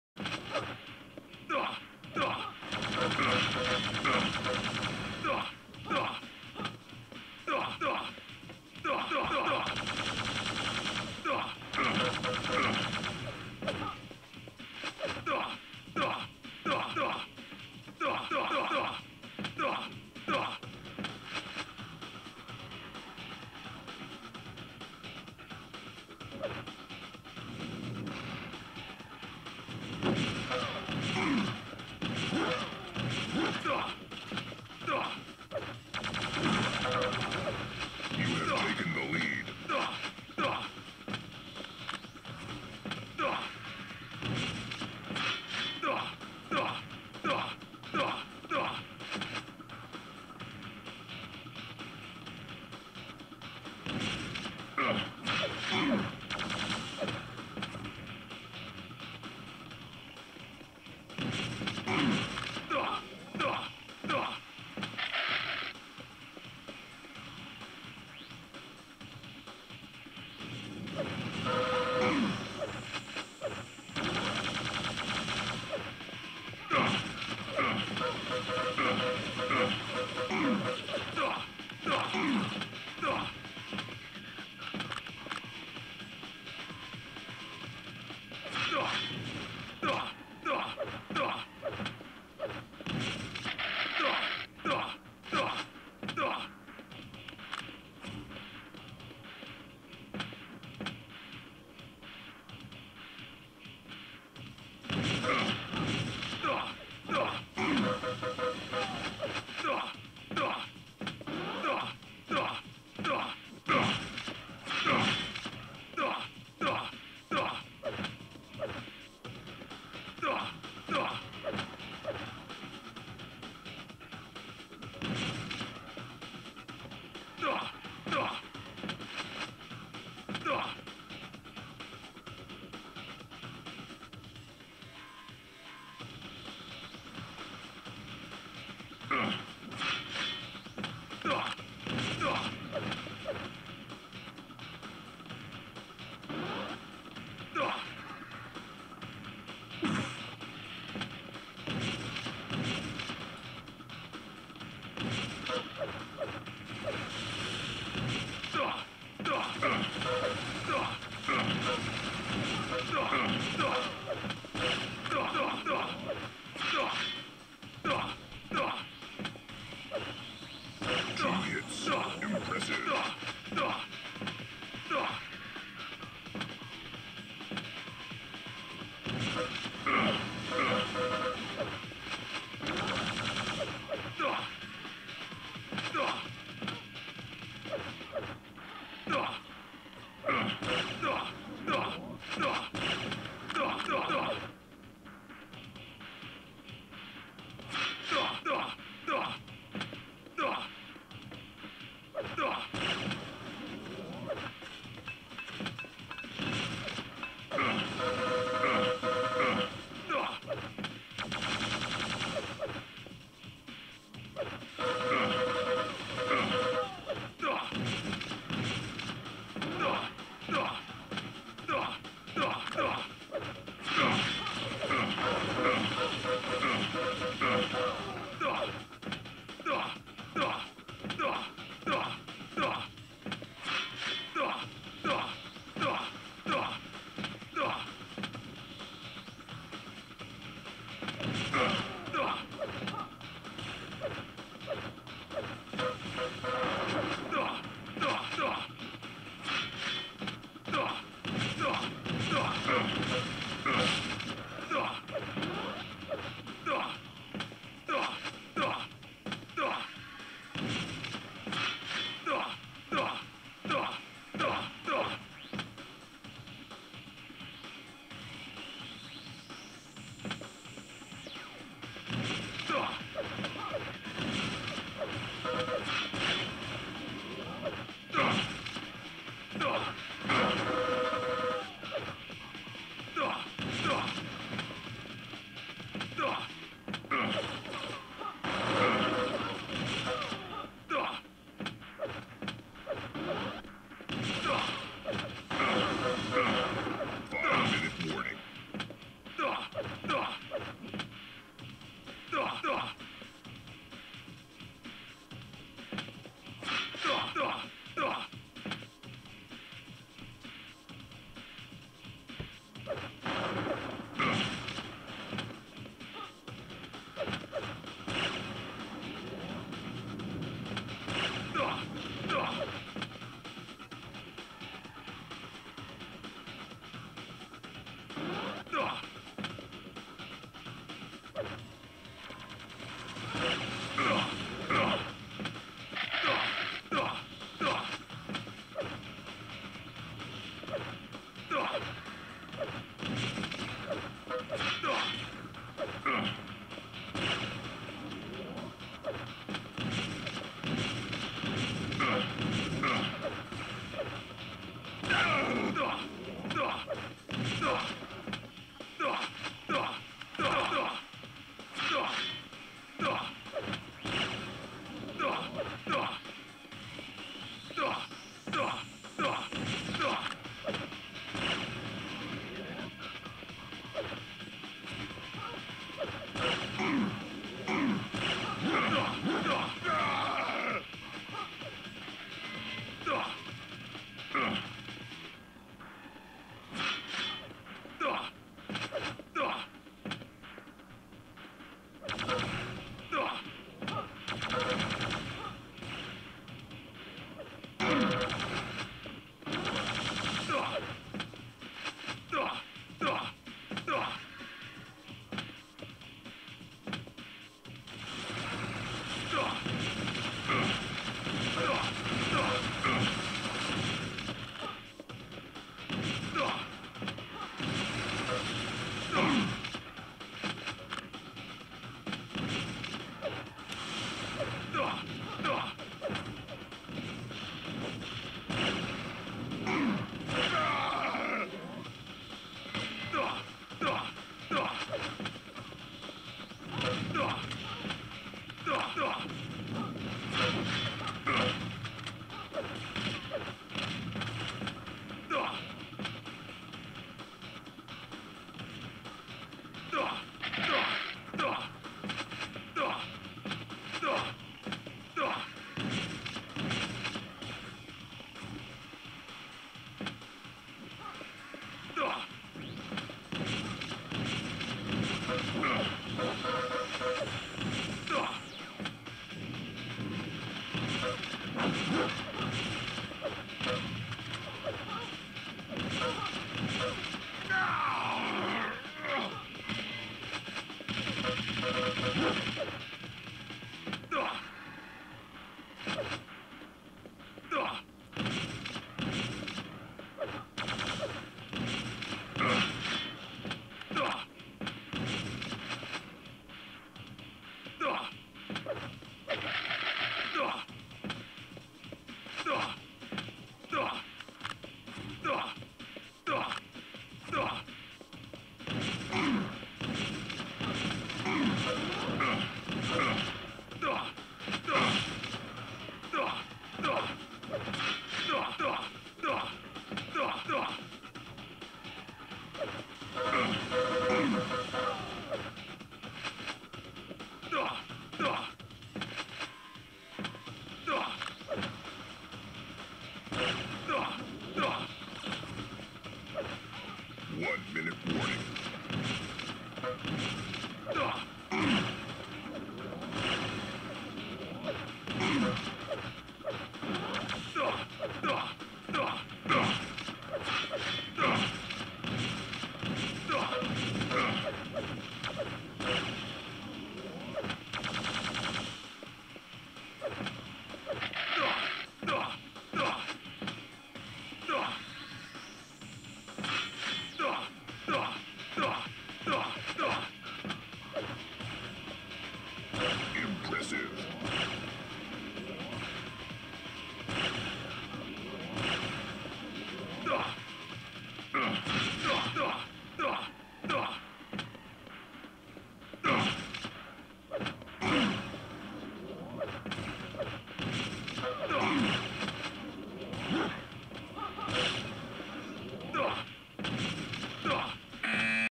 fnatic - spart1e vs. EG - DaHanG [game1]
This video is recorded from Spart1es POV. The match is from QuakeCon 2009, where he finished on second place.